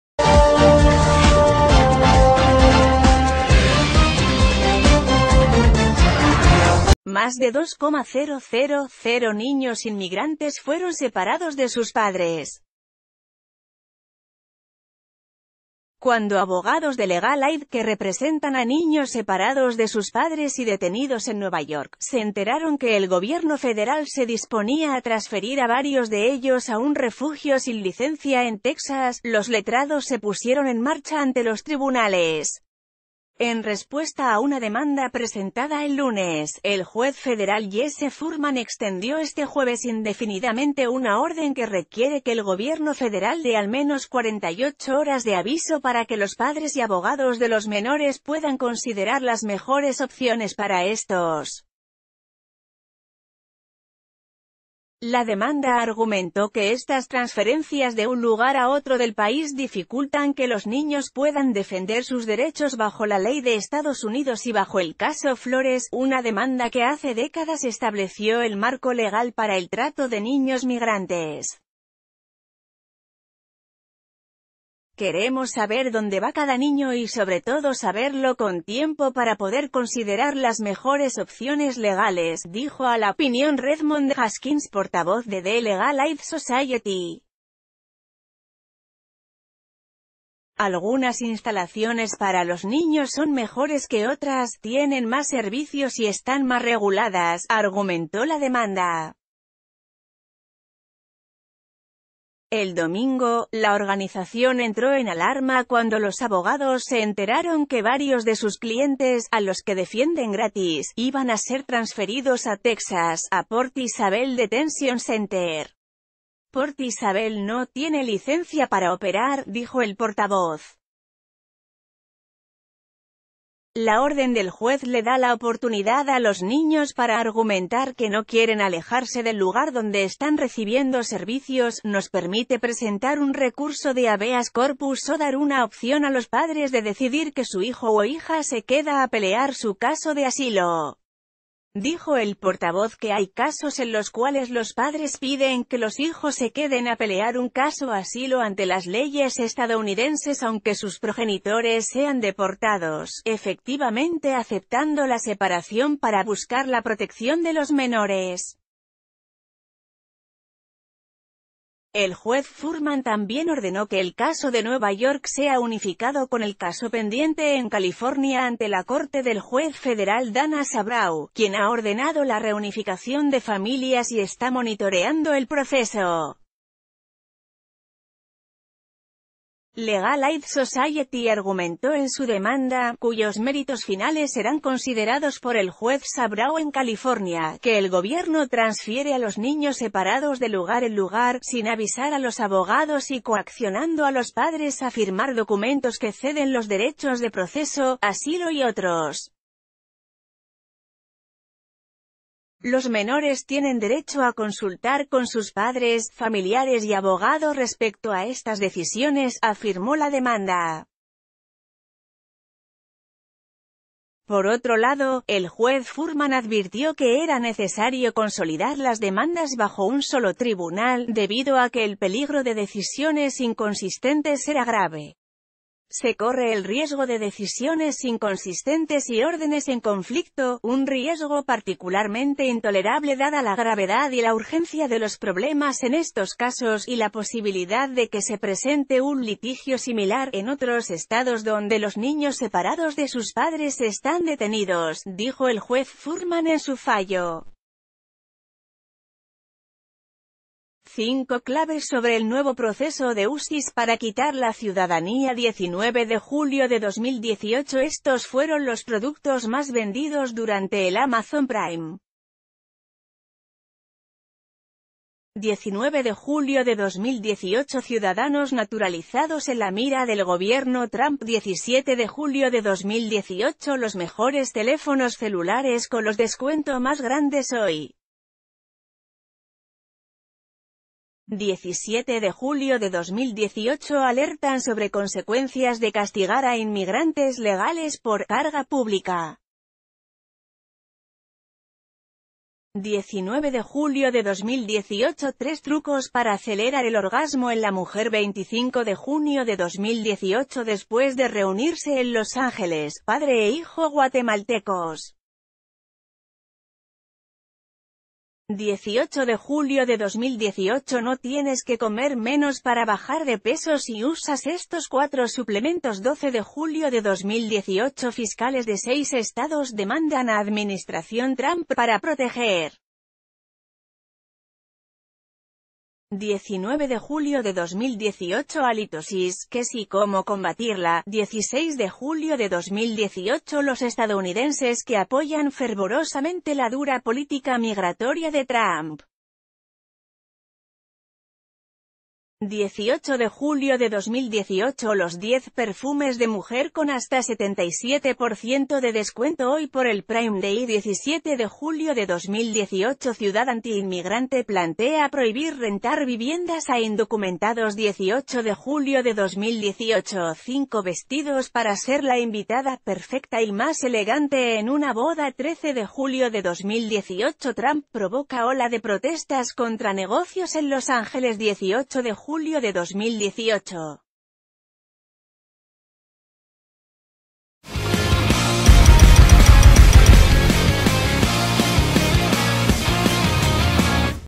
Juez ordena 48 horas de aviso antes de transferir a menores separados entre centros de detención
Más de 2,000 niños inmigrantes fueron separados de sus padres. Cuando abogados de Legal Aid que representan a niños separados de sus padres y detenidos en Nueva York, se enteraron que el gobierno federal se disponía a trasferir a varios de ellos a un refugio sin licencia en Texas, los letrados se pu...